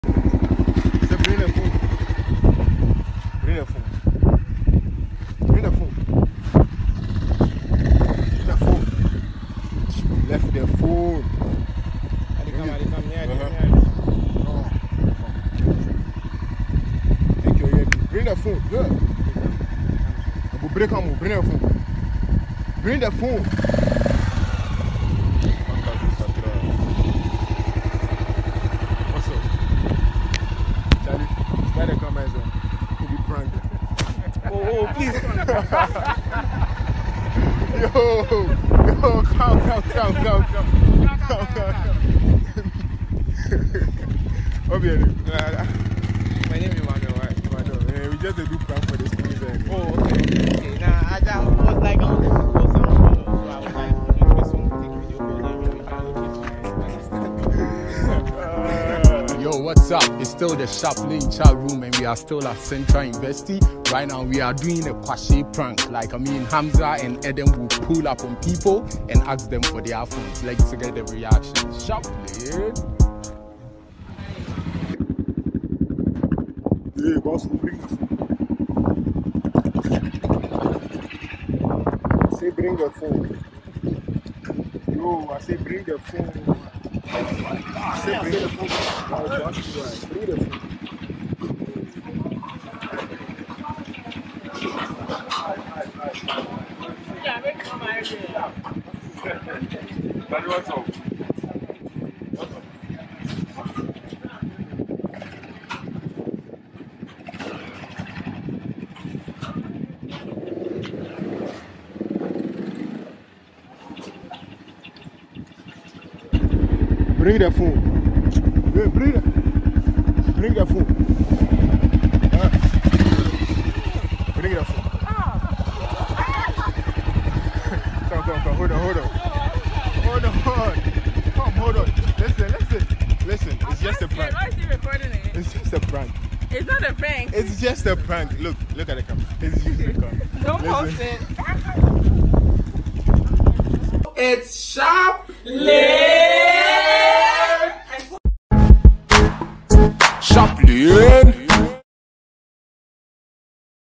Robbery Prank - Central University Edition - Sharp Lane Chat Room. Mobile Phone Robbery Prank
The Sharp Lane Chat Room is at Central University this week and we pranked a few students. This is a mobile phone Robbery Prank.